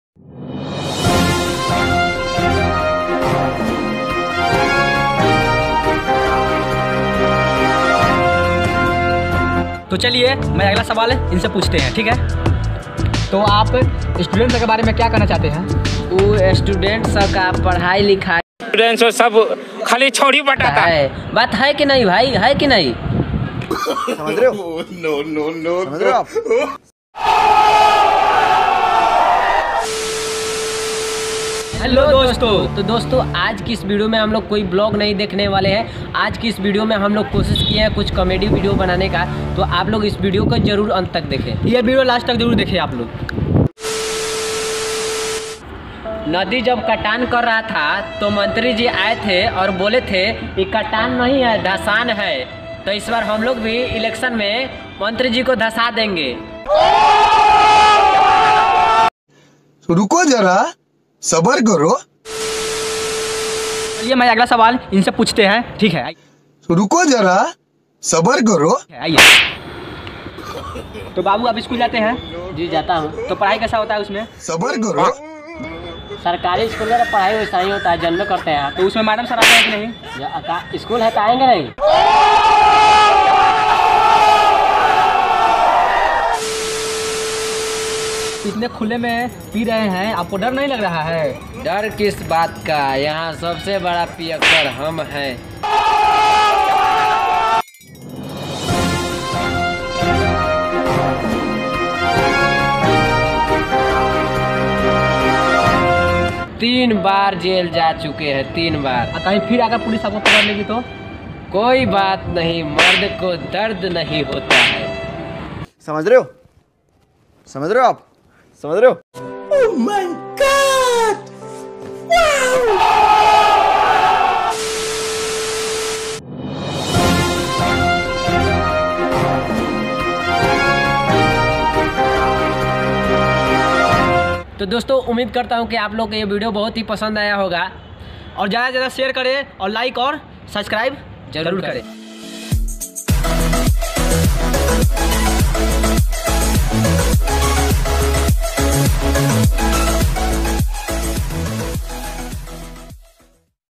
Khali Chhauri Patata Hai (Spoof ) l Funny News Interview l Biharwala Vlogs l Main Media Viral Video
In This video we are try to make some comedy video by taking some news media interested interview video.

Credit In this video
Main Media
The Lallantop
Biharwala Vlogs

Actor In This Video :
1.Nitish Kumar (Biharwala)
2.Rajkumar Sharma
3.Navnish Singh (Camera Man)

Script :
Biharwala Vlogs